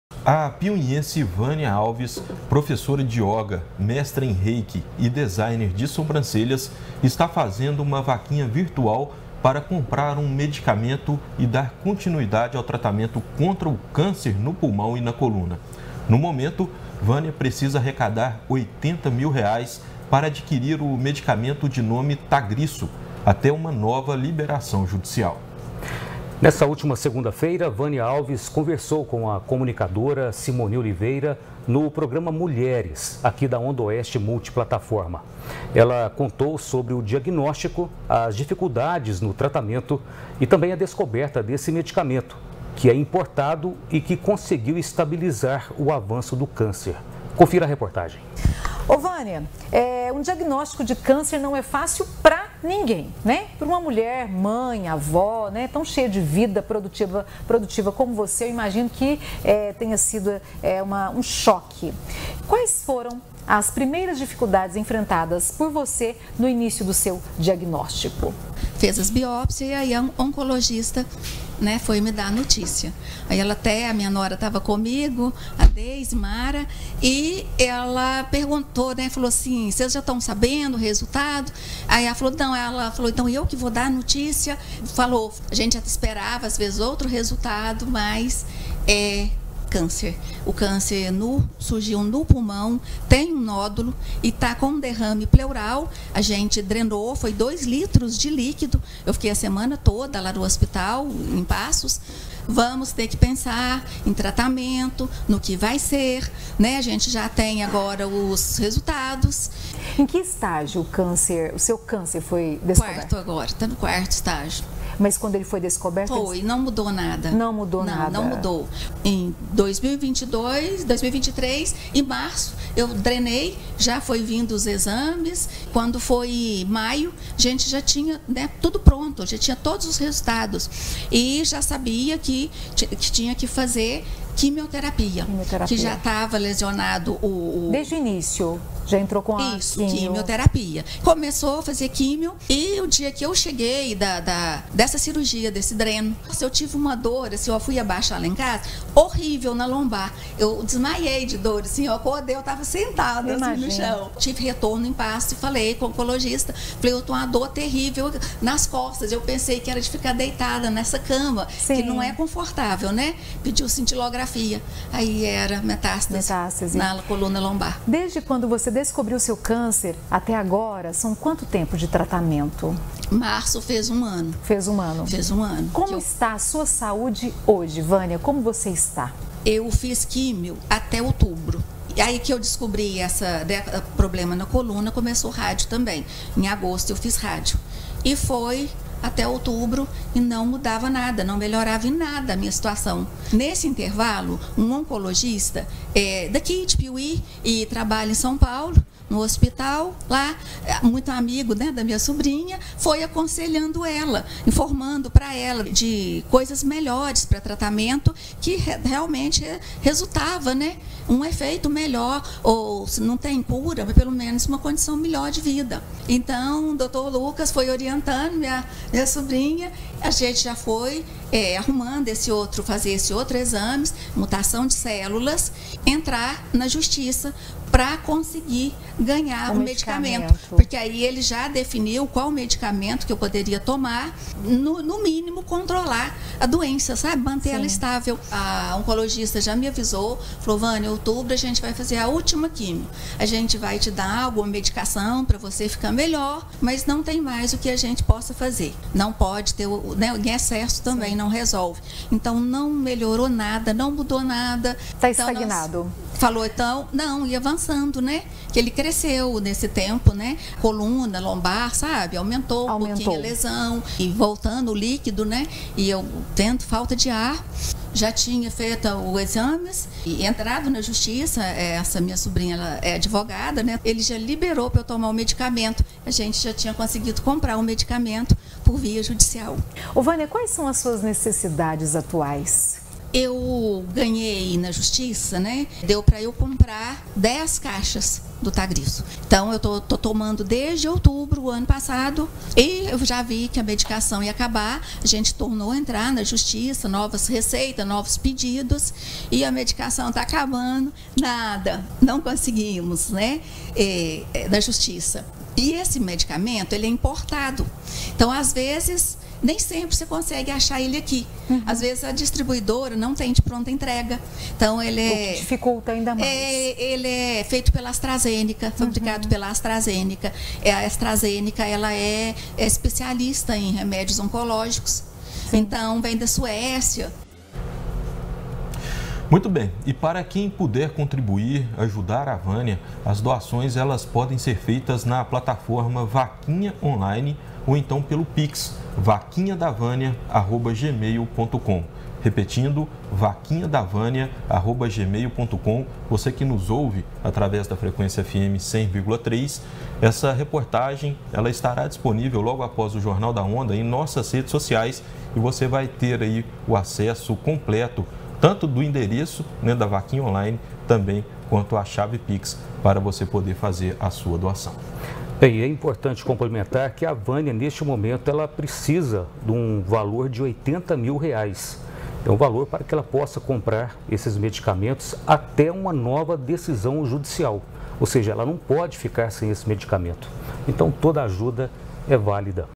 PIUMHIENSE VÂNIA ALVES PRECISA DE MEDICAMENTO PARA CONTINUAR TRATAMENTO CONTRA O CÂNCER
| PIUMHI |

A piumhiense Vânia Alves, professora de yoga, mestre em Reike e designer de sobrancelhas, está fazendo uma vaquinha virtual para comprar um medicamento e dar continuidade ao tratamento contra o câncer no pulmão e na coluna. No momento, Vânia precisa arrecadar 80 mil reais, para adquirir o medicamento de nome Tagrisso, até uma nova liberação judicial.

Nessa última segunda-feira, Vânia Alves conversou com a Simony Oliveira, no Programa Mulheres, aqui da Onda Oeste Multiplataforma. Ela contou sobre o diagnóstico, as dificuldades no tratamento e a descoberta desse medicamento, que é importado e que conseguiu estabilizar o avanço do câncer. Confira o trecho: